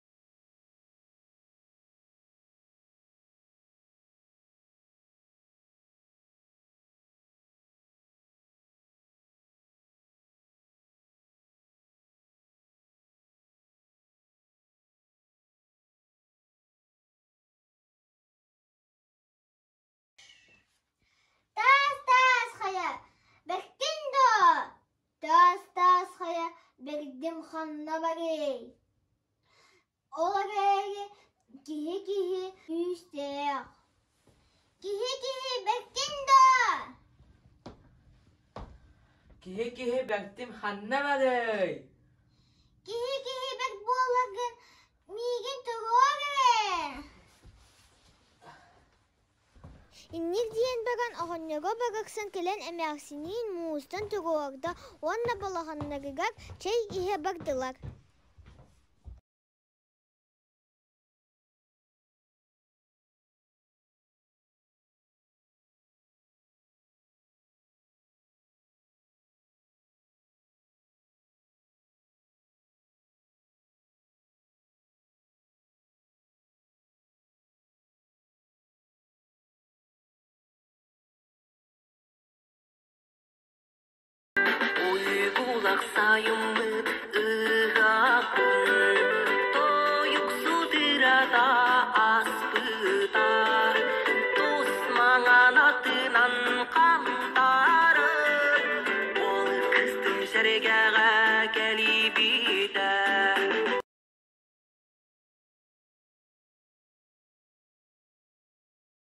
Горохова Юлиана,Горохова Алена Валерьевна
"Роль нашего дедушки и бабушки в воспитании  детей" .
"Роль нашего дедушки и бабушки в воспитании наших детей" . Выступление на НПК ЗУУМ по теме "Этнопедагогика" в честь дня рождения этнопедагога РФ Чиряева Константина Спиридоновича. Организатор ЯГНГ   ГО г.Якутск РС(Я).19 мая 2021 г.
Сказка "Таал-Таал".